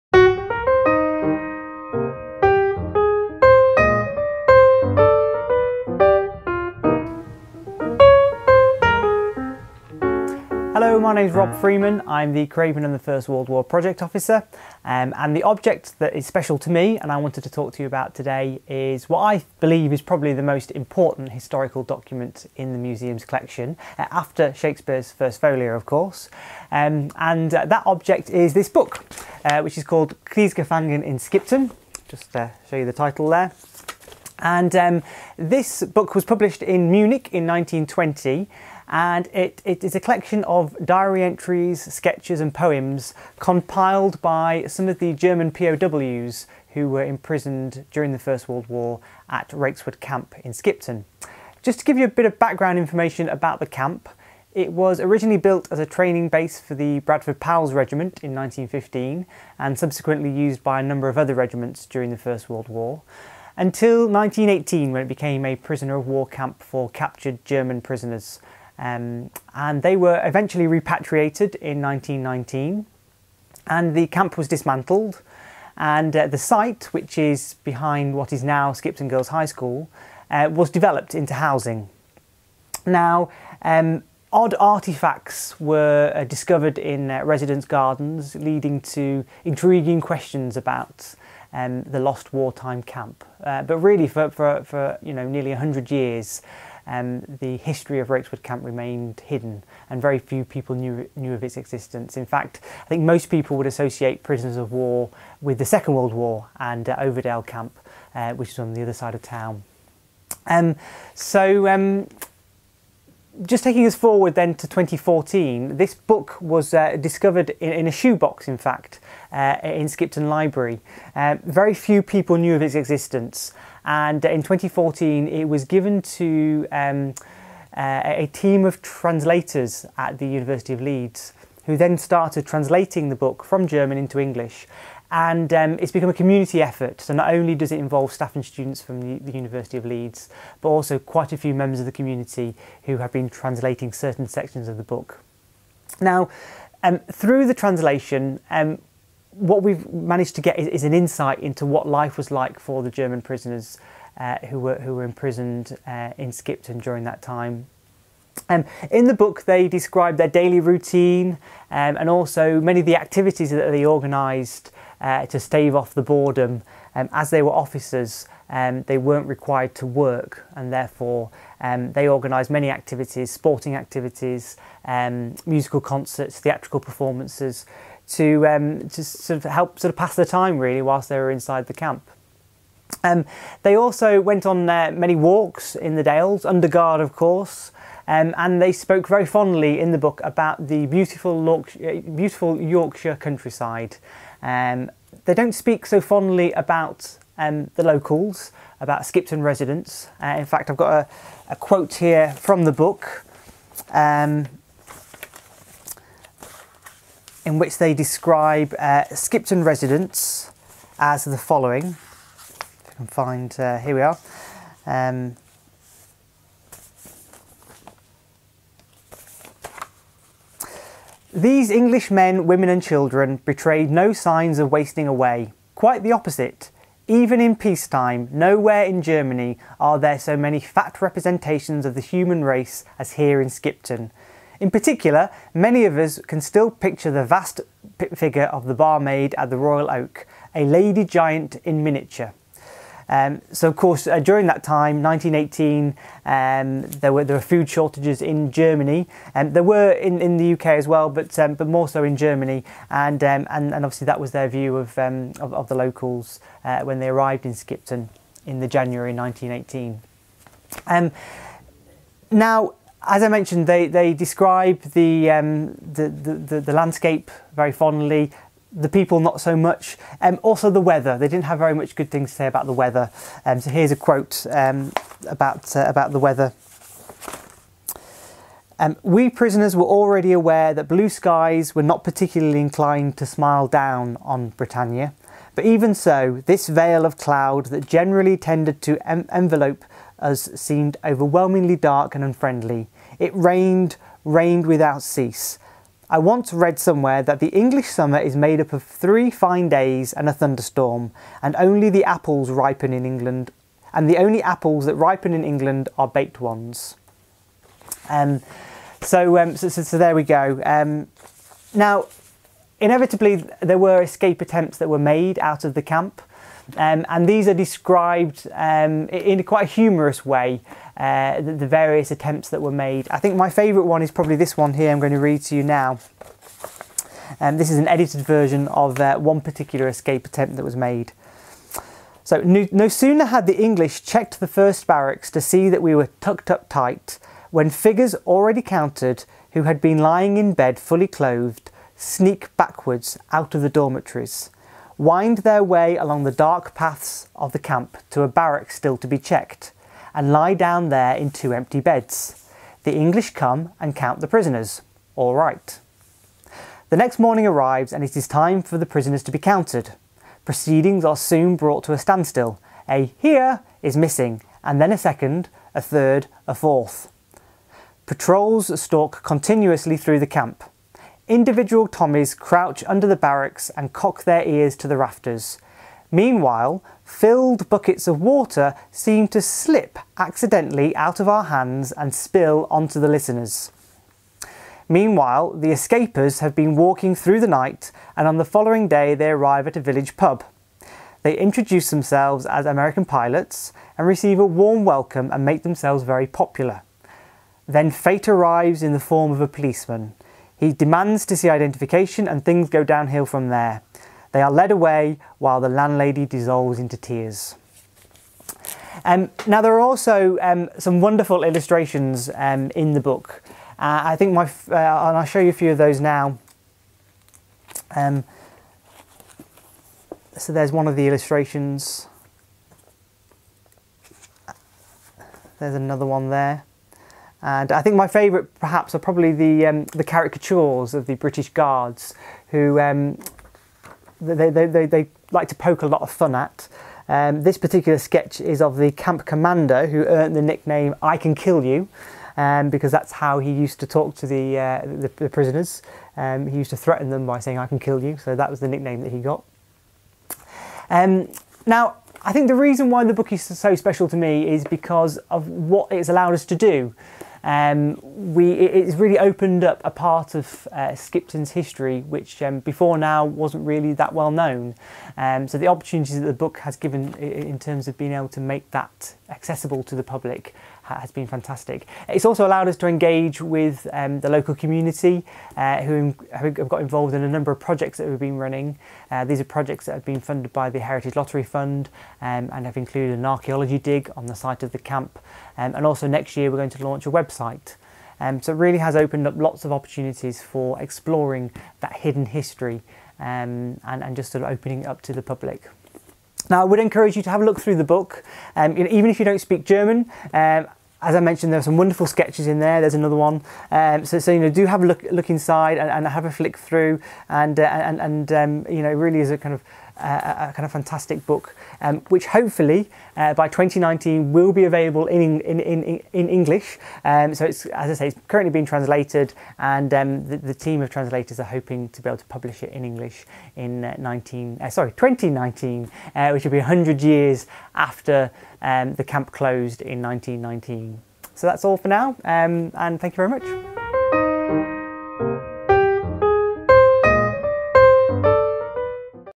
Object Jam - Raikeswood Camp
As part of 2017's Museums at Night celebrations, curators from Craven Museum and Gallery spoke about their favourite objects!

Rob Freeman, First World War Project Officer talks about a copy of the book 'Kriegsgefangen in Skipton'  written by German prisoners of war who were imprisoned at Raikeswood Camp in Skipton, North Yorkshire. The book gives fascinating insights to camp life, and the impressions that Skipton and Craven made.